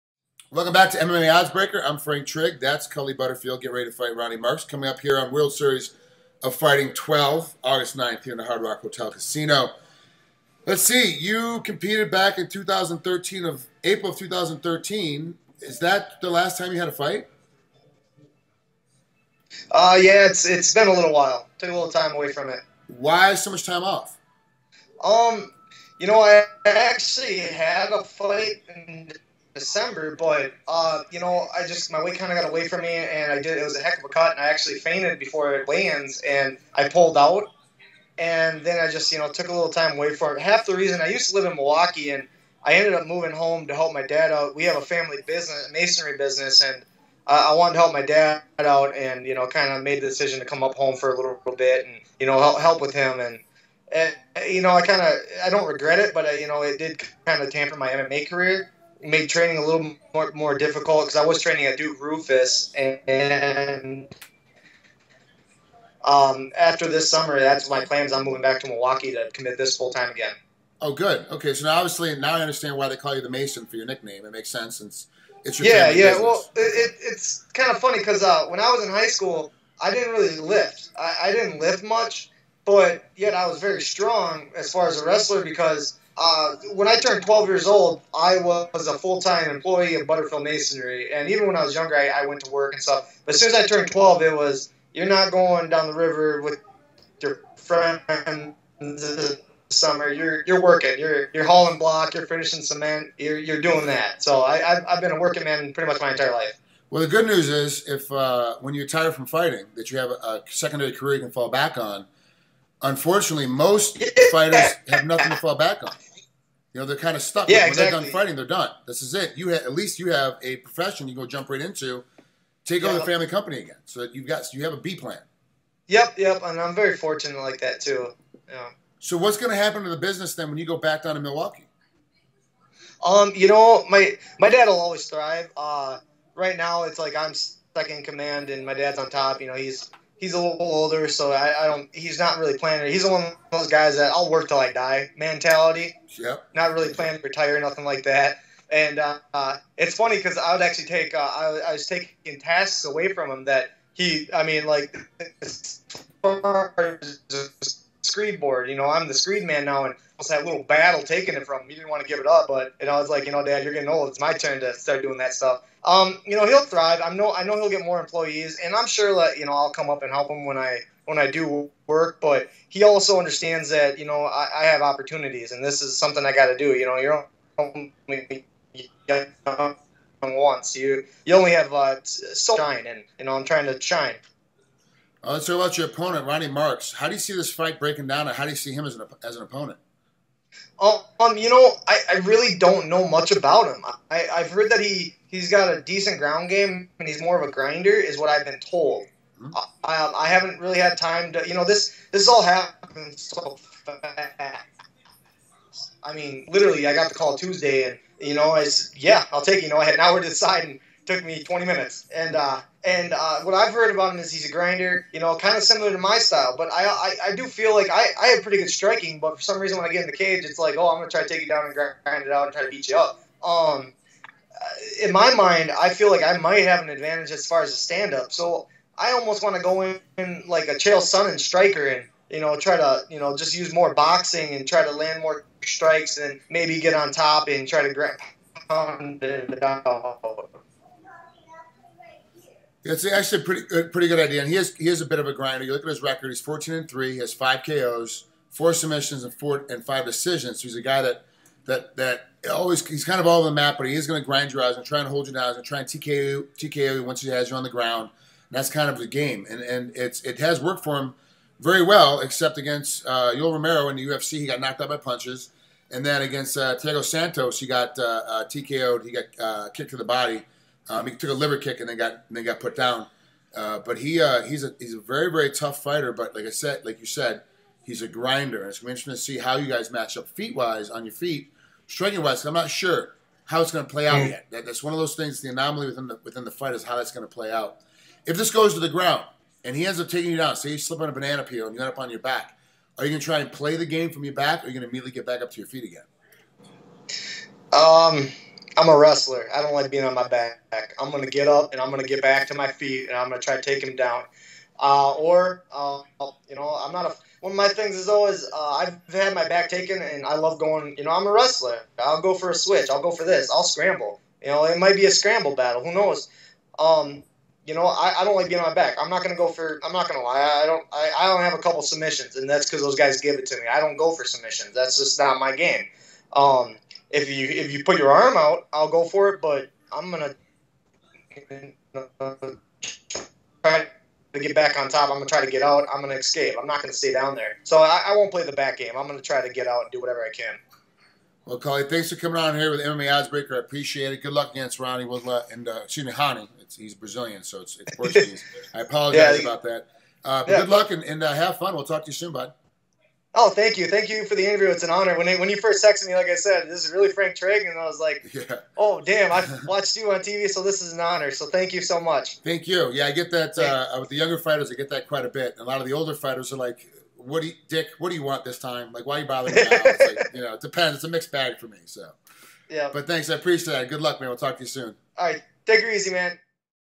WSOF 12's Cully Butterfield: 'I've got the better stand-up'
MMAOddsBreaker's Frank Trigg speaks with World Series of Fighting 12 middleweight Cully Butterfield as he prepares for his upcoming main card bout against Ronny Markes this Saturday (August 9, 2014) at the Hard Rock Hotel and Casino in Las Vegas, Nevada.